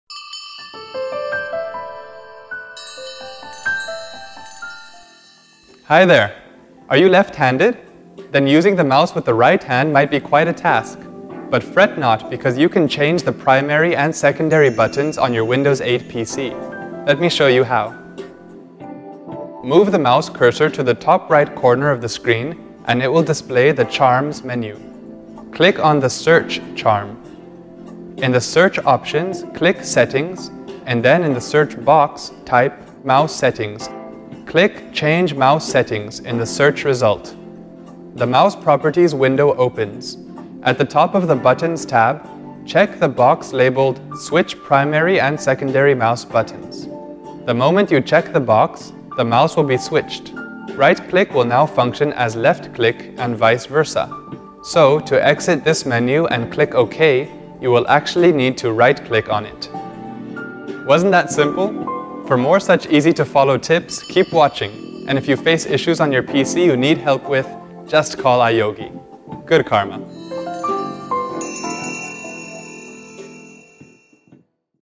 How to switch Mouse buttons in Windows® 8
In case you're left handed and want to switch the Mouse buttons in Windows® 8-based PC, here are the steps.